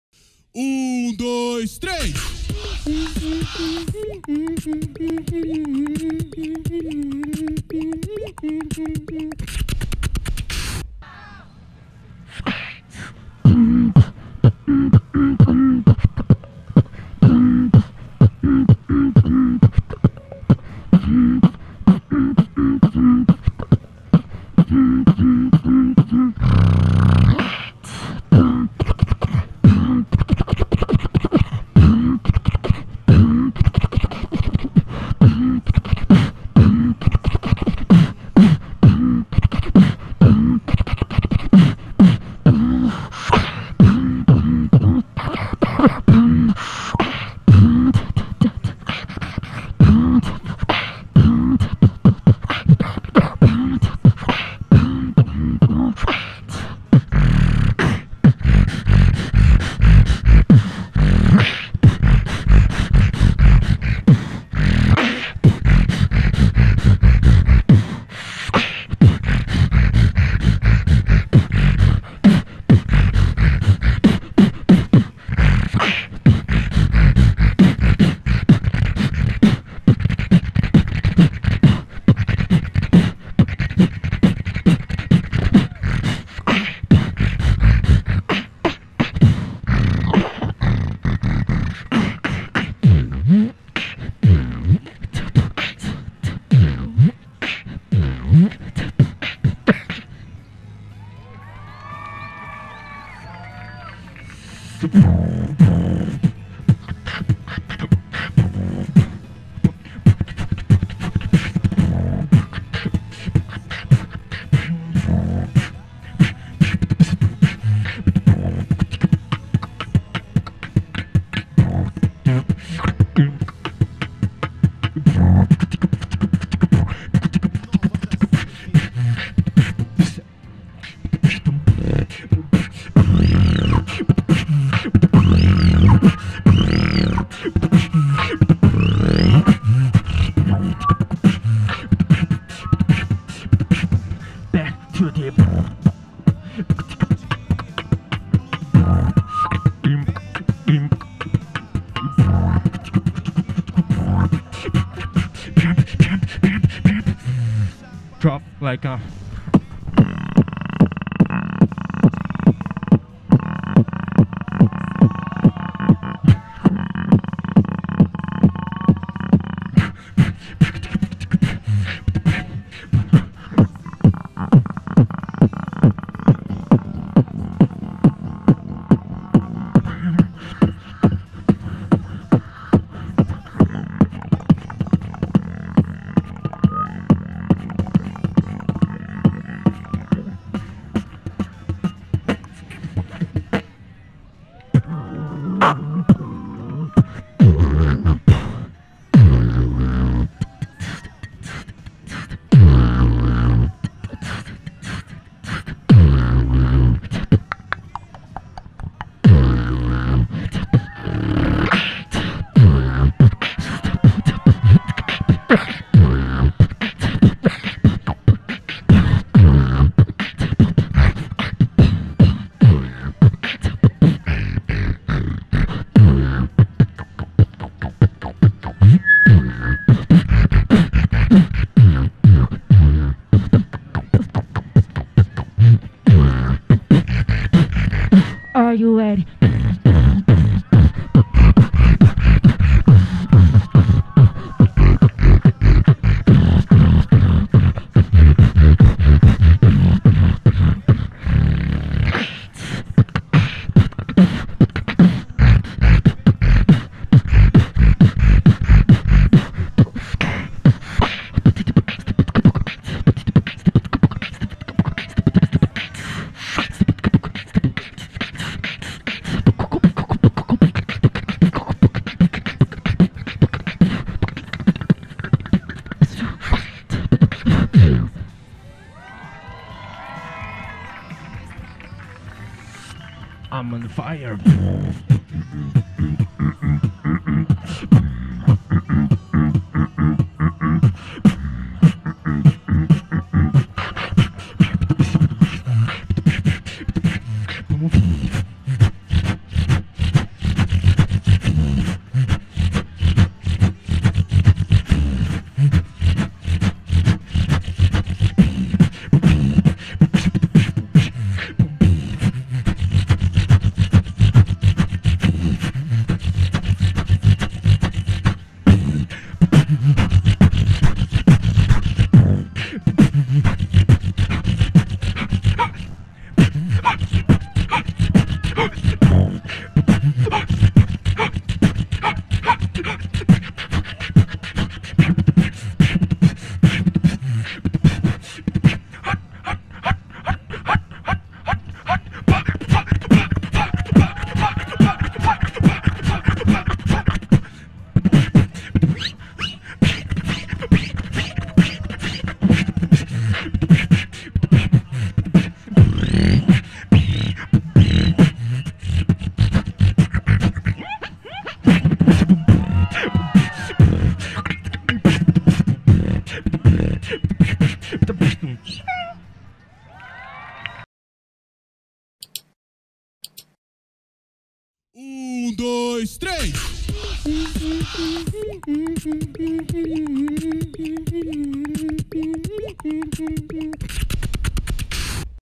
BeatMaster#64 | Jojo x Kbeats | BeatBox Nacional Br
Batalha realizada em Dezembro de 2019.

Salve!

A BeatMaster é a nosso formato mais clássico de batalha! 2 rounds pra mostrar ao público porque merece passar de fase.
Inscrições feitas na hora, só chegar!

Beatbox Nacional na cena!

Vencedora: Kbeats

Gosta de beatbox?